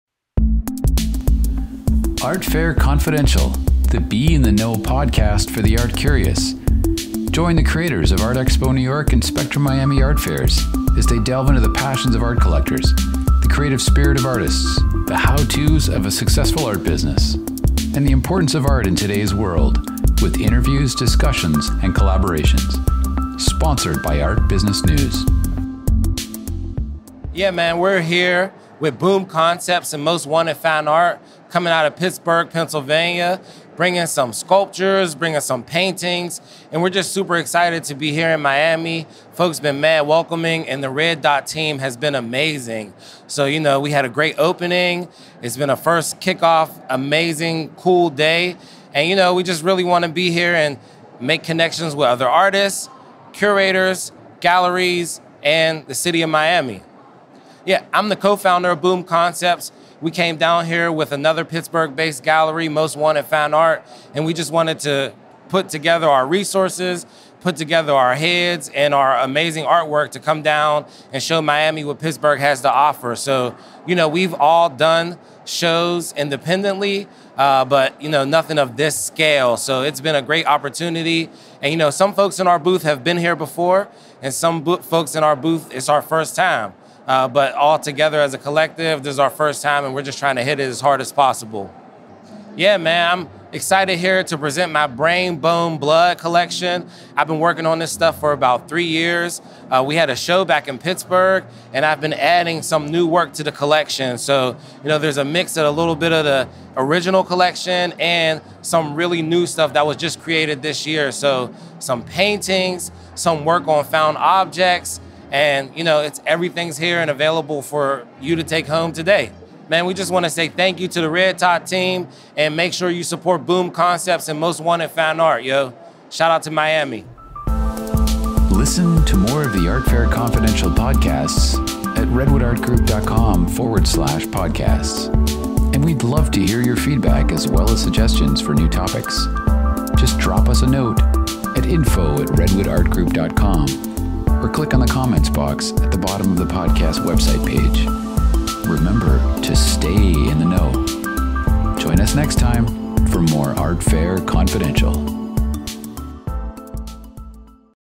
Red Dot 2023 - DS Kinsel
Red Dot Miami is a gallery-only contemporary art fair that takes place annually as part of the popular Miami Art Week.